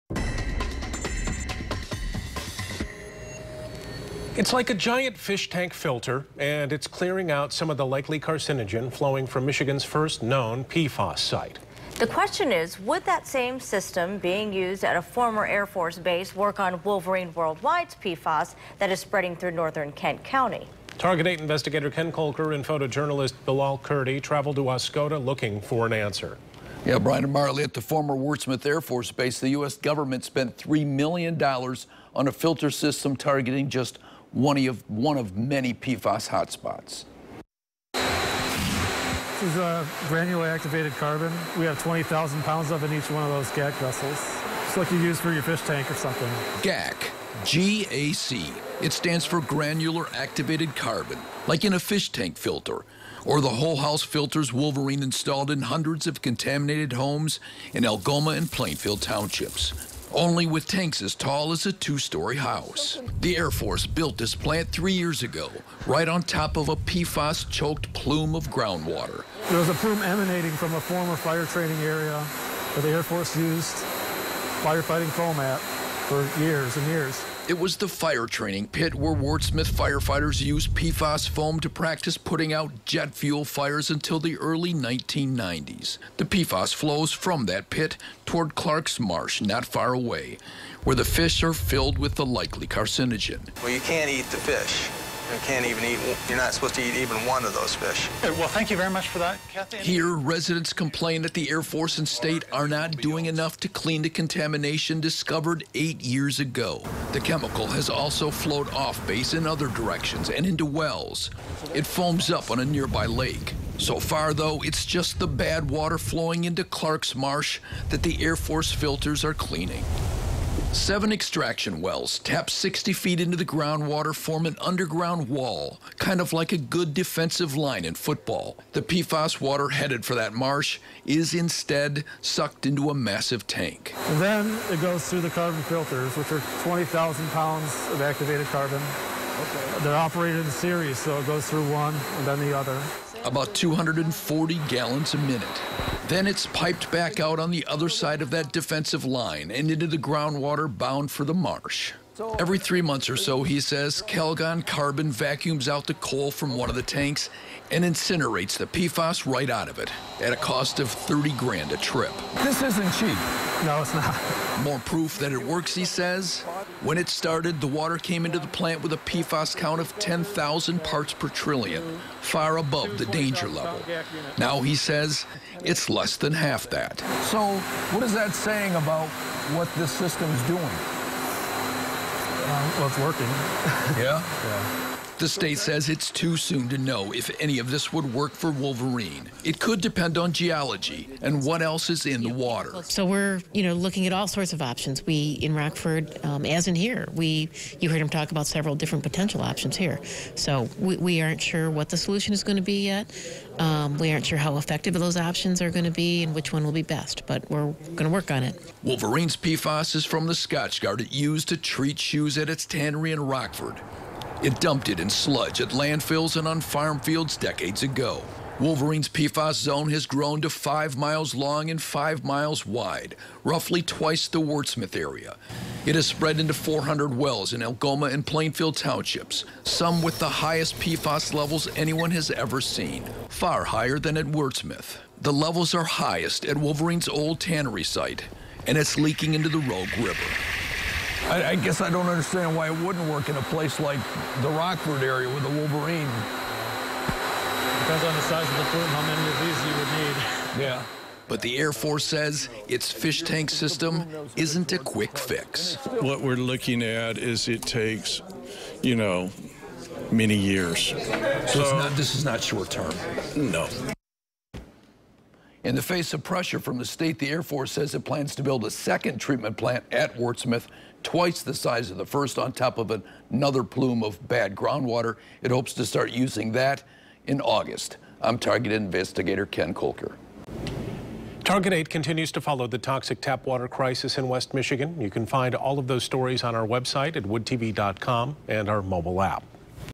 Giant 'fish tank' filters clean PFAS from MI's first known site
It works like a giant fish tank filter, and it's clearing out some of the likely carcinogen from Michigan's first known PFAS site. The question is whether the system being used at the former Wurtsmith Air Force Base in Oscoda, near Lake Huron, would work on Wolverine Worldwide's PFAS spreading through northern Kent County. (Feb. 20, 2018)